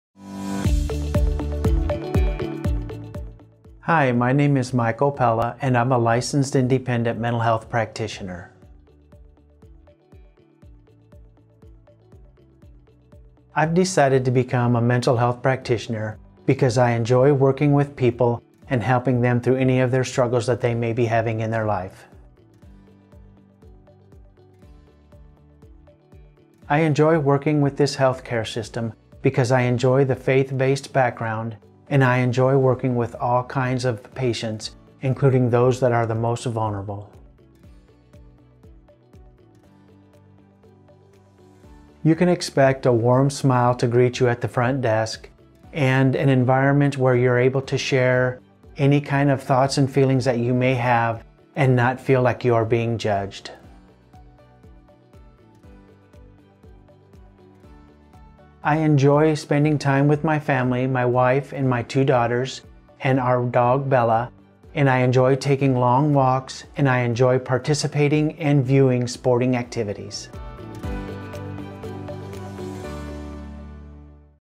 Michael Pella, LIMHP - Mental Health Therapy - CHI Health
Michael Pella is a provider in Mental Health Therapy at CHI Health.